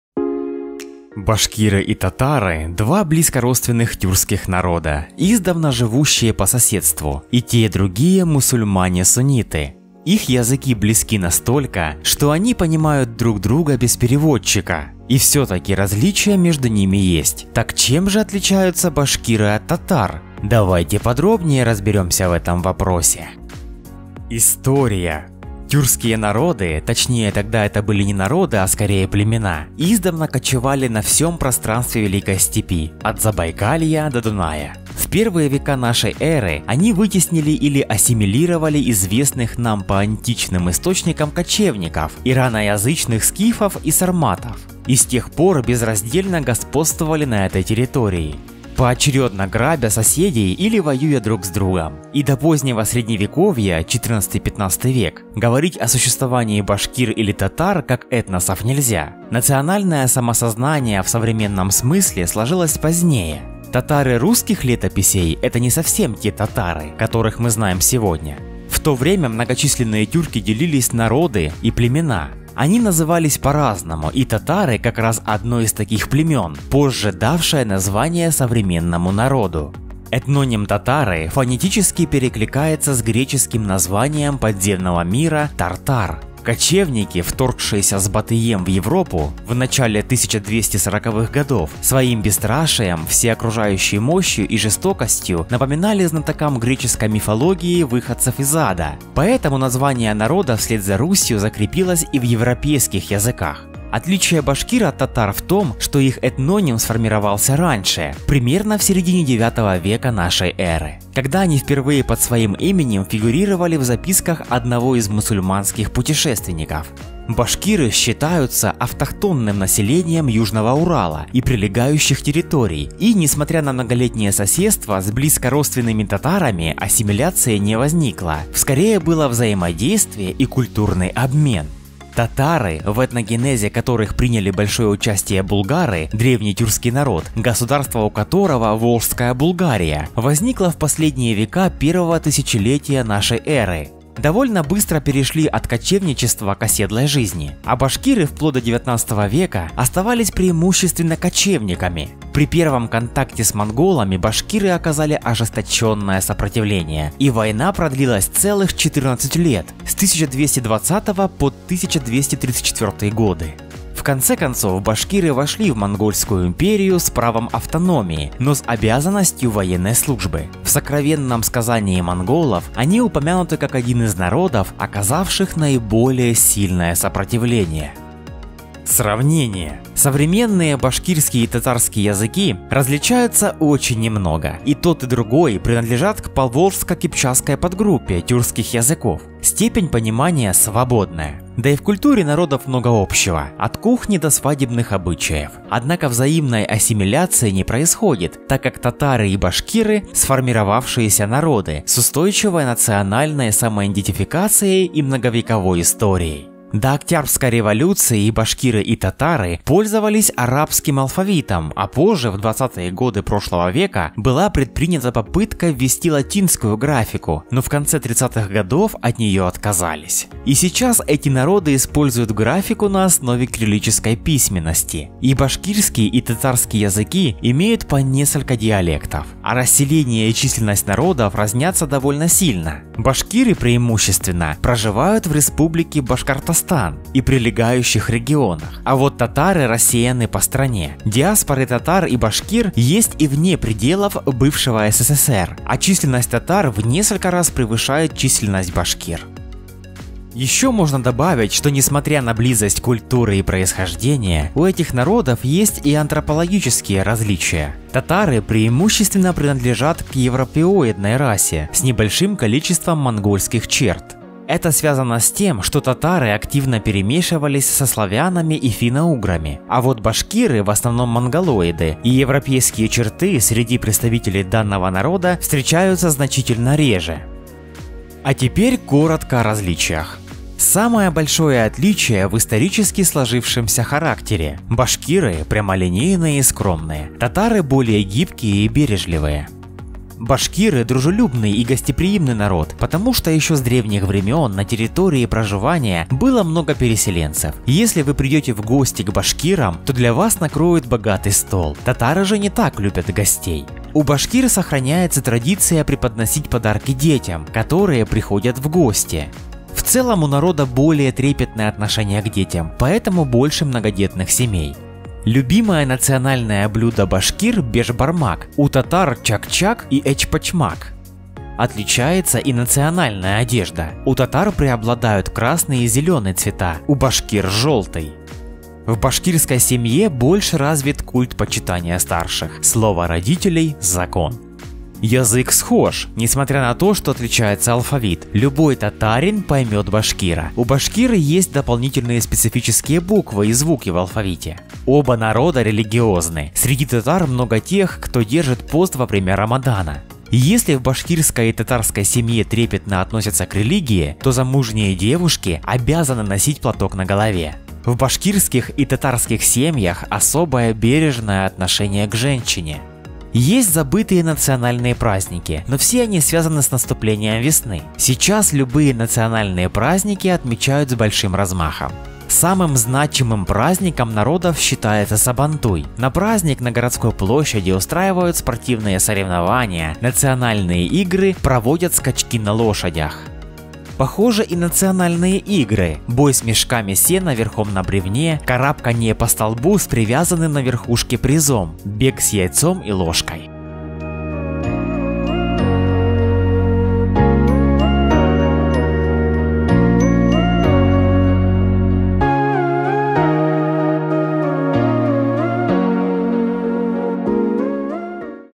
Чем отличаются татары от башкир?
Так в чём же различия татар от башкир? Подробнее рассказываю в видео.